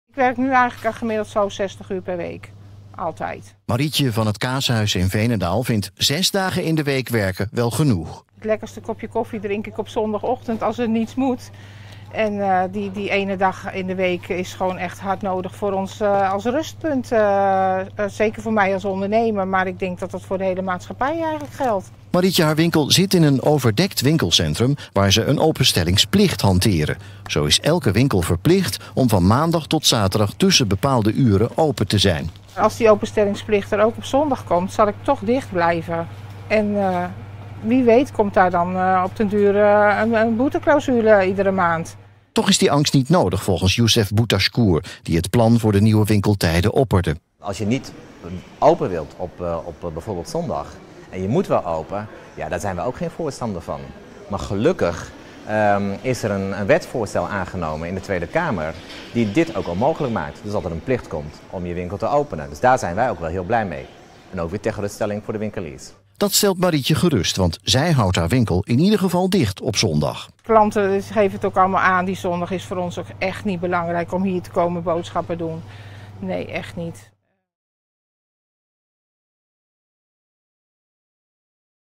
Veenendaal verruimt openingstijden voor winkels, ook op zondag [RTV Utrecht]
De koopzondag in de gemeente Veenendaal wordt verruimd. Een meerderheid van de gemeenteraad stemde gisteren in met een voorstel van D66 dat het mogelijk maakt dat ondernemers zelf kunnen bepalen of ze open gaan of niet.

"Waarom mag een Praxis wel open op zondag en de Leen Bakker niet? Waarom mag de Albert Heijn wel open en een drogisterij als Kruidvat niet?" zei raadslid Youssef Boutachekourt bij aanvang van het debat. D66 diende het voorstel in en kreeg de steun van VVD, ProVeenendaal, CDA, Lokaal Veenendaal, DENK, GroenLinks, PvdA en Fractie Saidi.

De verruiming van de openingstijden voor nachtwinkels als onderdeel van het voorstel kwam ook ter sprake, maar dit onderdeel haalde het niet. Volgens onder meer de PvdA zorgen nachtwinkels voor overlast in de omgeving.